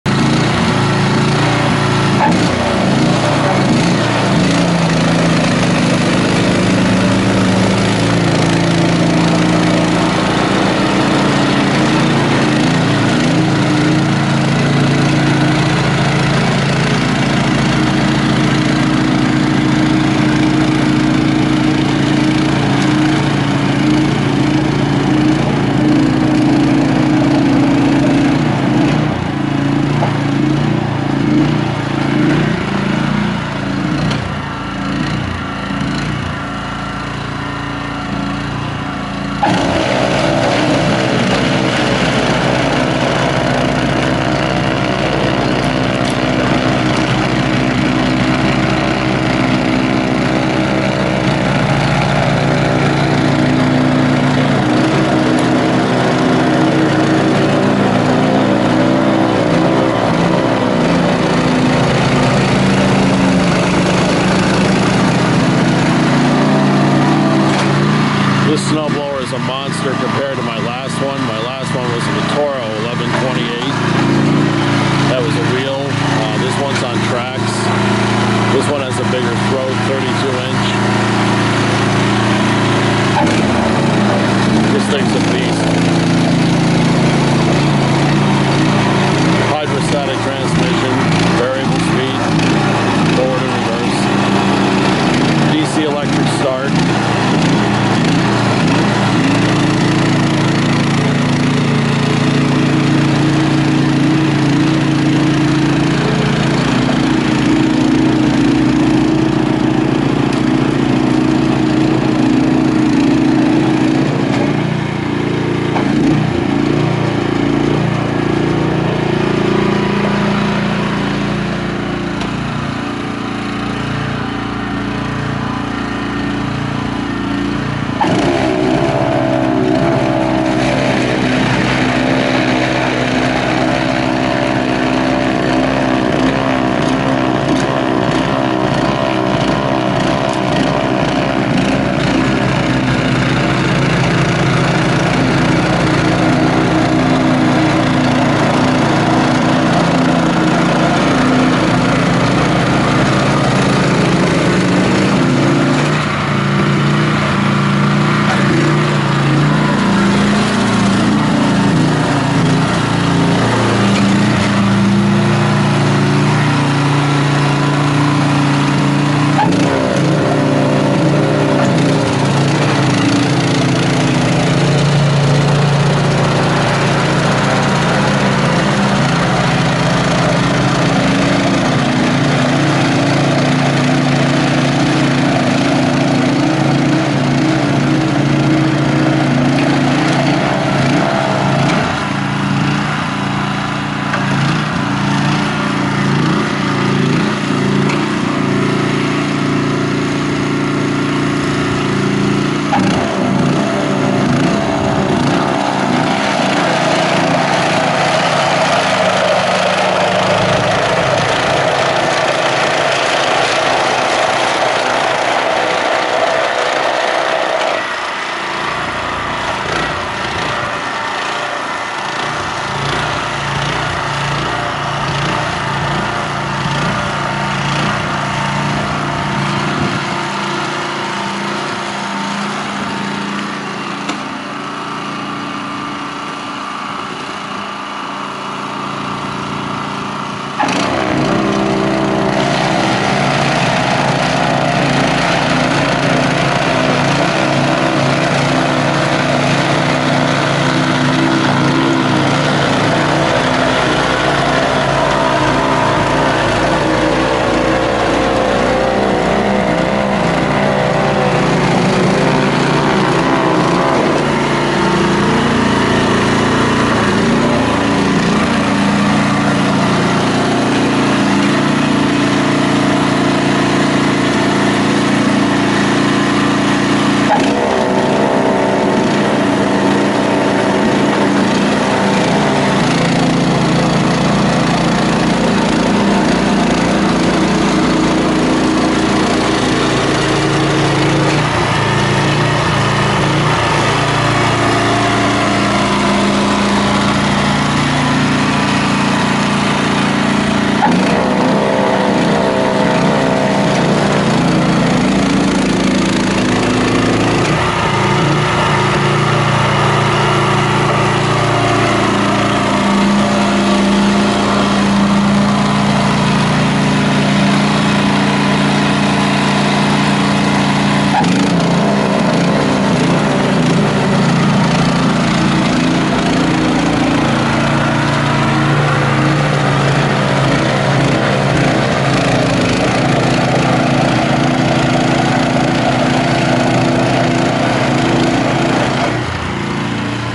Video #7 2016 Honda HSS1332ATD snowblower in April 4th snow storm
April 4th, 2016 in upstate NY we got 4.5 inches of wet snow. The biggest snow accumulation we have gotten all year. Finally got to test out the new 2016 Honda HSS1332ATD snowblower (even after thinking I never would use it this year) and it worked great! Great machine!
Check back next season, can't wait to get some big snow storms next year and use it and get some video's of it in the deep powder!